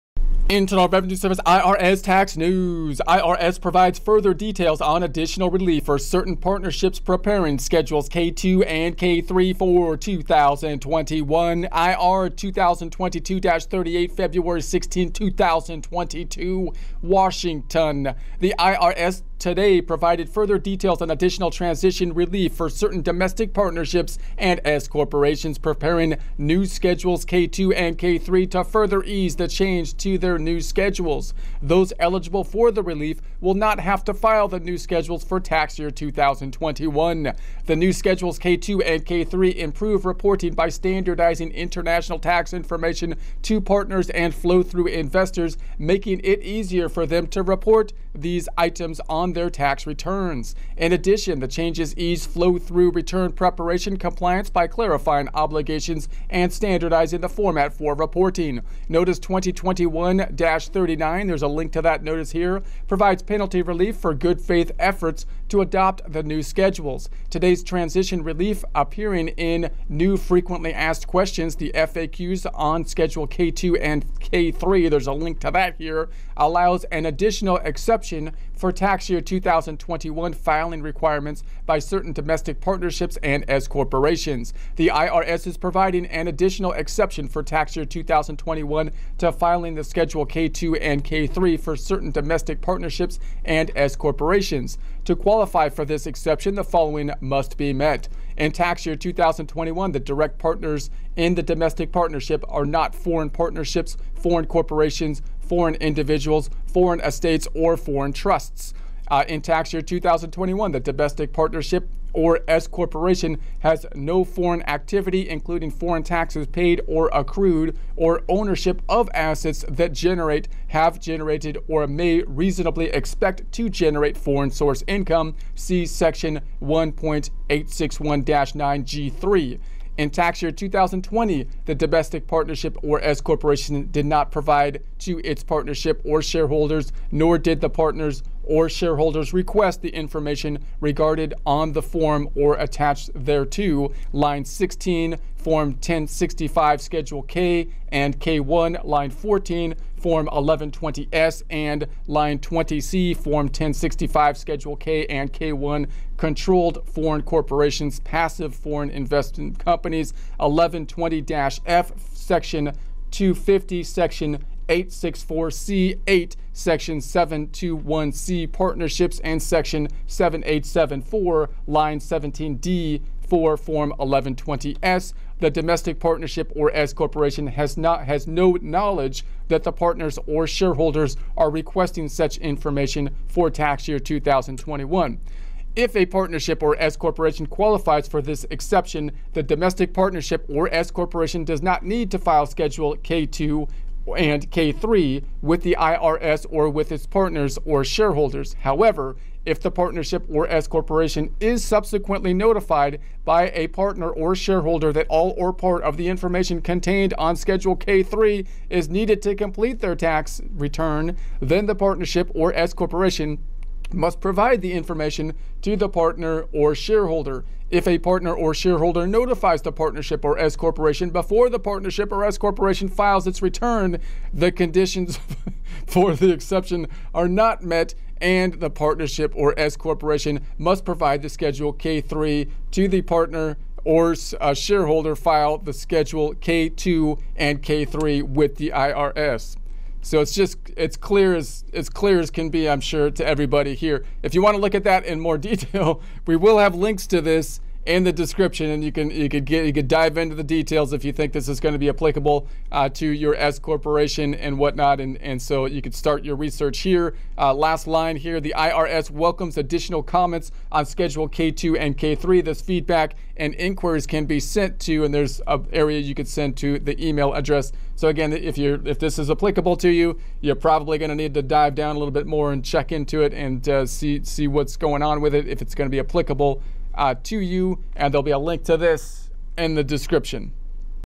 IRS provides further details on additional relief for certain partnerships preparing schedules 104
IRS provides further details on additional relief for certain partnerships preparing schedules K-2 and K-3 for 2021
The IRS today provided further details on additional transition relief for certain domestic partnerships and S corporations preparing the new schedules K-2 and K-3 to further ease the change to these new schedules. Those eligible for the relief will not have to file the new schedules for tax year 2021.

The new schedules K-2 and K-3 improve reporting by standardizing international tax information to partners and flow-through investors, making it easier for them to report these items on their tax returns. In addition, the changes ease flow-through return preparation compliance by clarifying obligations and standardizing the format for reporting.

Notice 2021-39 PDF provides penalty relief for good-faith efforts to adopt the new schedules. Today's transition relief, appearing in new frequently asked questions (FAQs) on Schedules K-2 and K-3, allows an additional exception for tax year 2021 filing requirements by certain domestic partnerships and S corporations.

The IRS is providing an additional exception for tax year 2021 to filing the Schedules K-2 and K-3 for certain domestic partnerships and S corporations. To qualify for this exception, the following must be met:

In tax year 2021, the direct partners in the domestic partnership are not foreign partnerships, foreign corporations, foreign individuals, foreign estates or foreign trusts.
In tax year 2021, the domestic partnership or S corporation has no foreign activity, including foreign taxes paid or accrued or ownership of assets that generate, have generated or may reasonably expected to generate foreign source income (see section 1.861-9(g)(3)).
In tax year 2020, the domestic partnership or S corporation did not provide to its partners or shareholders nor did the partners or shareholders request the information regarding (on the form or attachments thereto):
Line 16, Form 1065, Schedules K and K-1 (line 14 for Form 1120-S), and
Line 20c, Form 1065, Schedules K and K-1 (Controlled Foreign Corporations, Passive Foreign Investment Companies, 1120-F, section 250, section 864(c)(8), section 721(c) partnerships, and section 7874) (line 17d for Form 1120-S).
The domestic partnership or S corporation has no knowledge that the partners or shareholders are requesting such information for tax year 2021.
If a partnership or S corporation qualifies for this exception, the domestic partnership or S corporation does not need to file Schedules K-2 and K-3 with the IRS or with its partners or shareholders. However, if the partnership or S corporation is subsequently notified by a partner or shareholder that all or part of the information contained on Schedule K-3 is needed to complete their tax return, then the partnership or S corporation must provide the information to the partner or shareholder. If a partner or shareholder notifies the partnership or S corporation before the partnership or S corporation files its return, the conditions for the exception are not met and the partnership or S corporation must provide the Schedule K-3 to the partner or shareholder and file the Schedules K-2 and K-3 with the IRS.

The IRS welcomes additional comments on Schedules K-2 and K-3.